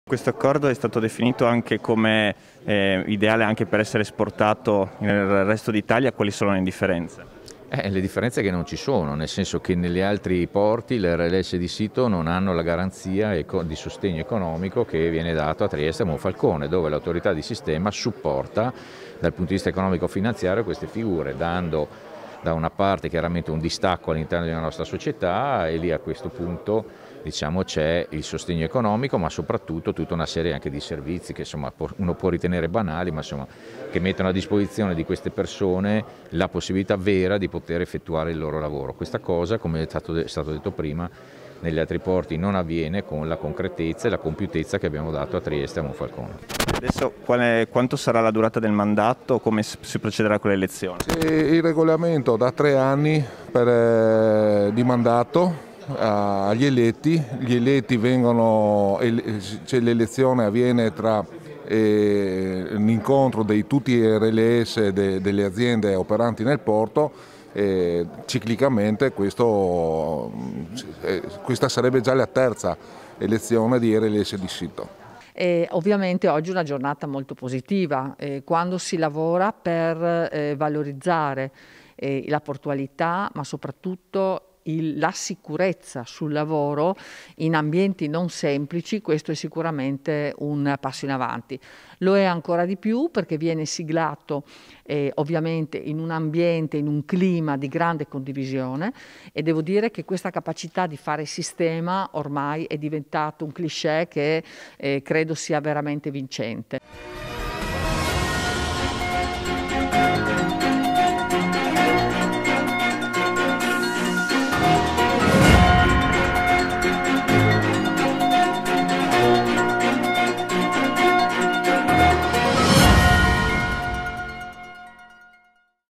«Più sicurezza nel porto di Monfalcone», accordo tra Autorità e sindacati
Sindacati e Autorità portuale fanno squadra per accrescere la sicurezza in cantiere a Monfalcone. Questa mattina, il Prefetto di Gorizia Raffaele Ricciardi ha ospitato la firma del protocollo per individuare il Rappresentante dei lavoratori per la sicurezza di sito produttivo (Rlss), che sarà eletto tra i rappresentanti dei lavoratori per la sicurezza (Rls) delle singole imprese che operano nell’area. Figure che già sono presenti nella vicina Trieste, puntando così a rendere il più possibile omogenea la struttura organizzativa tra le due realtà.